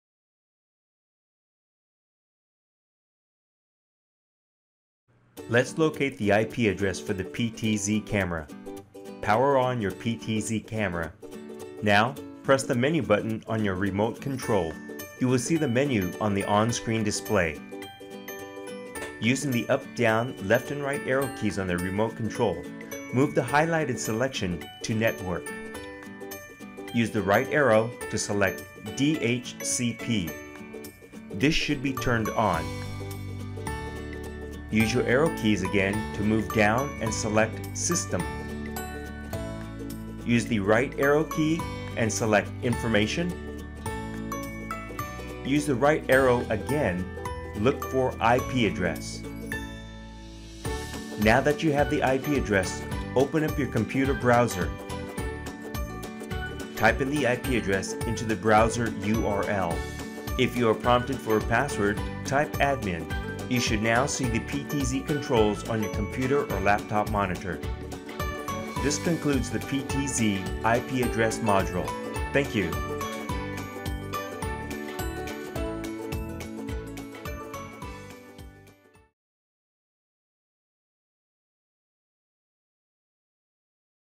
PTZ IP Address Setup
An overview of how to locate your IP address on an AVer PTZ camera.